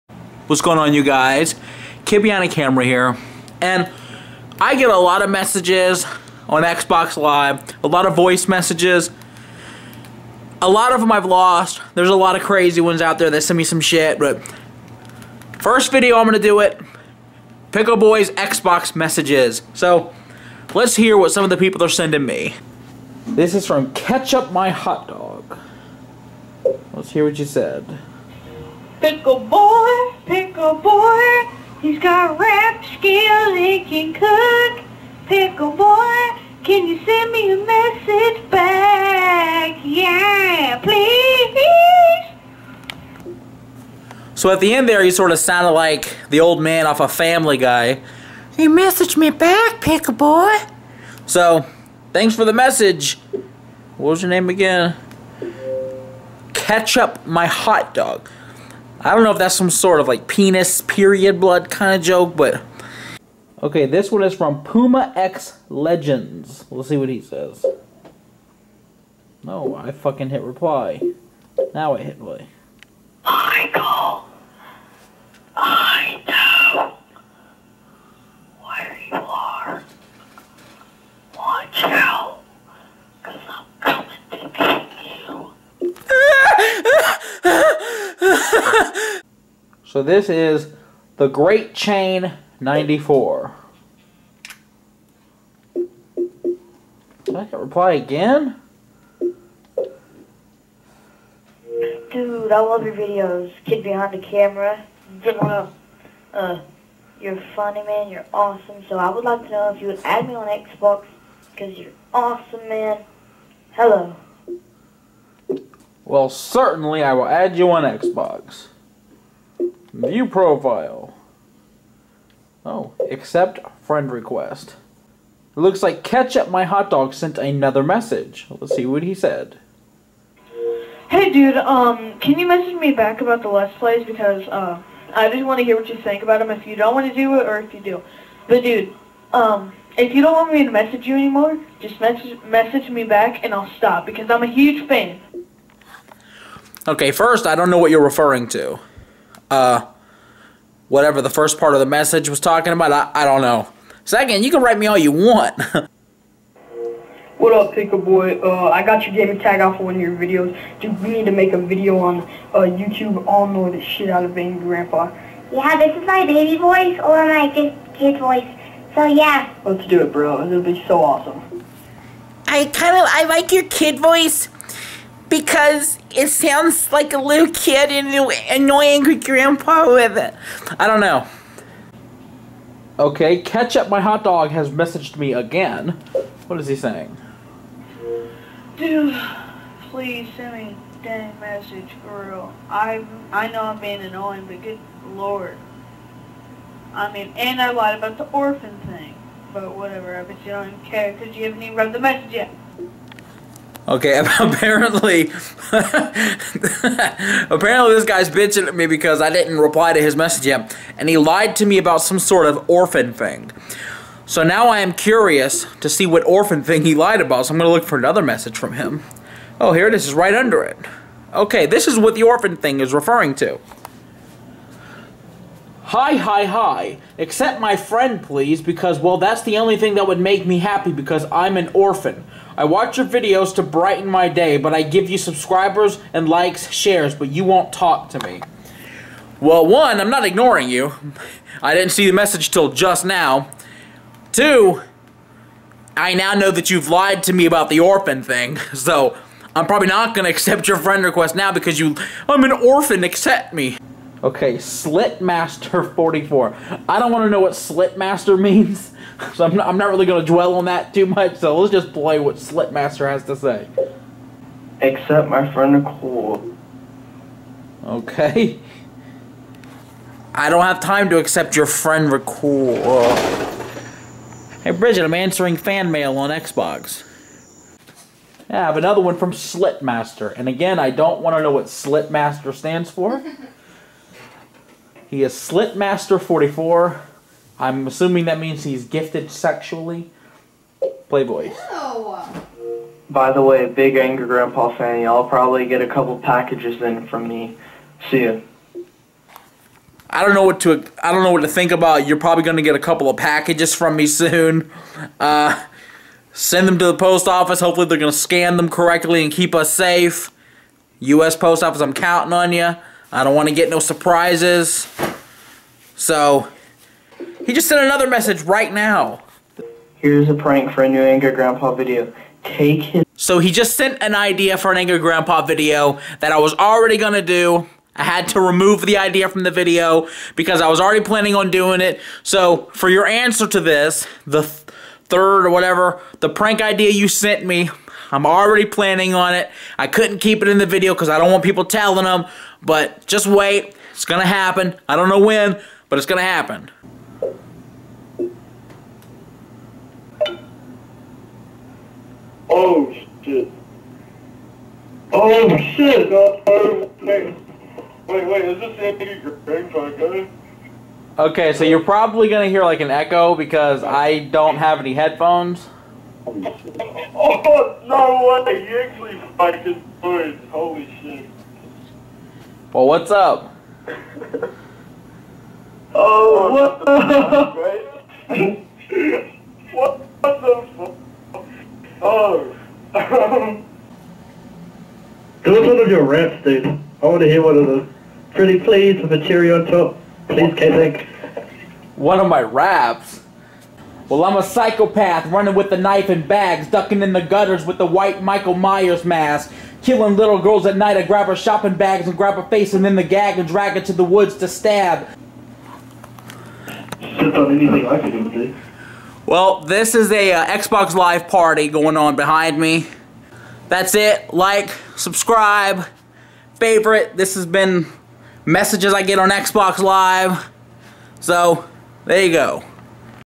Xbox Live Viewer Mail
Checking out some of the messages I receive on Xbox Live